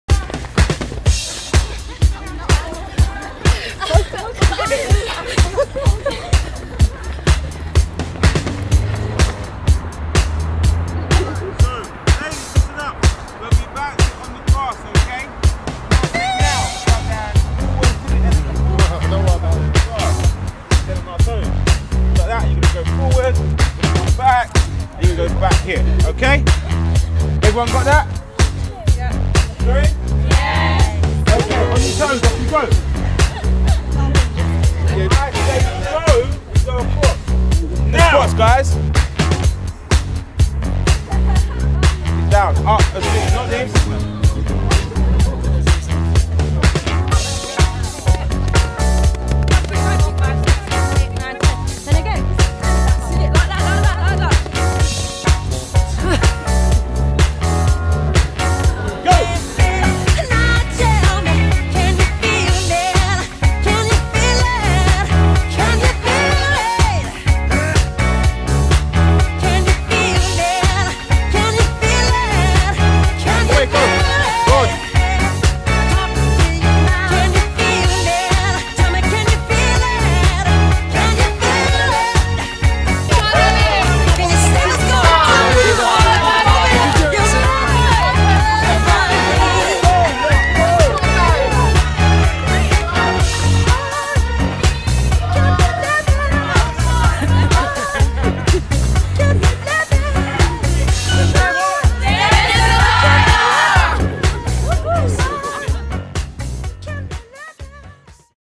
Dennis & Dyer Boxing Academy Loughton - Ladies Class
F A R· K I N G ·F I L M  -  PRESENTS - THE LADIES - Promo shot for @Dennis&dyerba in Loughton Essex. Had a lot of fun creating this for the gym, they really love what they do and they do it well! Make contact to discuss a promotional video for you or your business.